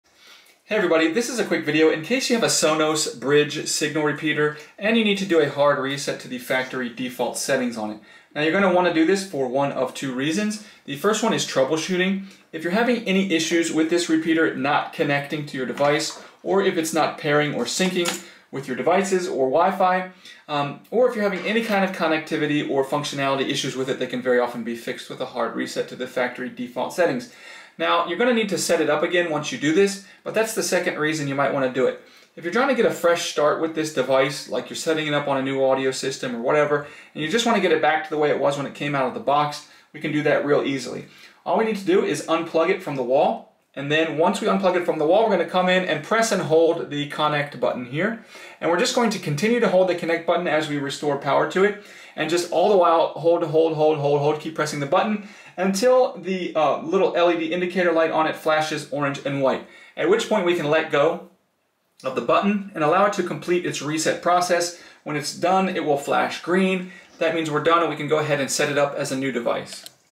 Hard Reset Sonos Bridge Repeater
How to troubleshoot or setup starting with a hard reset a Sonos Bridge Repeater to factory default settings if it is not working or not connecting or if it is frozen or not responding, the factory reset is a good troubleshooting step. You can also restore the factory settings to setup your device as new. Found in the Sonos Bridge Repeater user manual.